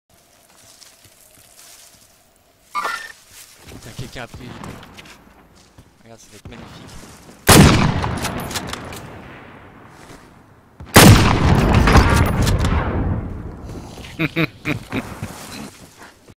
Hold, no scope chopper down ? what ?
Bobbytrapping is so satisfaying :) easiest no scope ever

This Clip is direct Submisson (especially for @MrJCorbs and Lottie) for any compilation as long as you credit "po0psandwich"
 link to my tiktok or Youtube channel are appreciated.